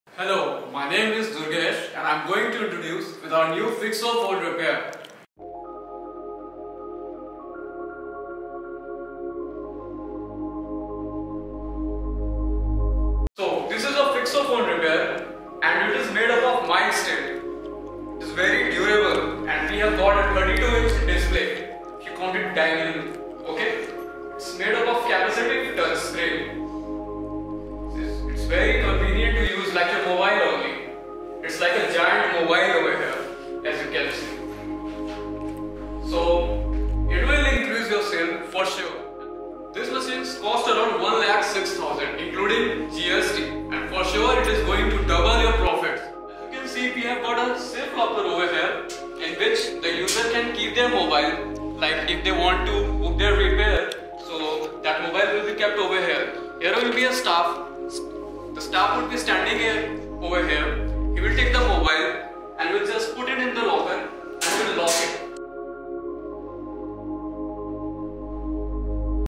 Phixo Phone Repair | KIOSK Phixman | Manual Phone Repair
In this video, you will get to know more about the Phixo Phone Repair KIOSK.
The Phixo phone repair is a self-service inquiry KIOSK with customizable Graphical User Interfaces for various applications. The user may inquire about desired information regarding their Gadget problems or any other phone troubles by simply touching the screen, enjoying convenient and fast services. They are very easy to use and can help you book your Phone Repair from anywhere they are installed if you cannot make it visit your nearest Phixman store for getting your Gadgets Repaired. It can be established in malls, metro stations, and other similar locations. Convenience like no other!